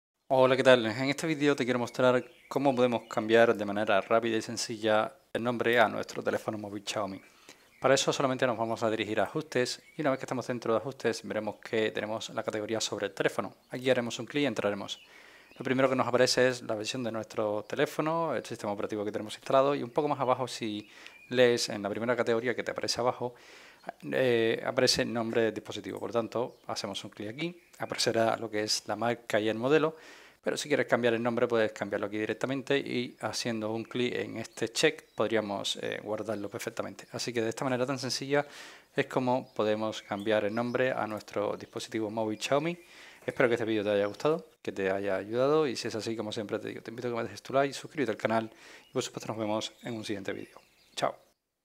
📱 Cambiar el Nombre de tu Teléfono Xiaomi: Personaliza tu Dispositivo | Xiaomi HyperOS
¿Quieres personalizar tu experiencia con Xiaomi? Aprende a cambiar el nombre de tu teléfono móvil para darle un toque único. En este tutorial, te mostramos paso a paso cómo cambiar el nombre de tu dispositivo Xiaomi para identificarlo fácilmente en redes Wi-Fi y dispositivos Bluetooth. ¡Haz que tu Xiaomi sea verdaderamente tuyo con estos simples pasos!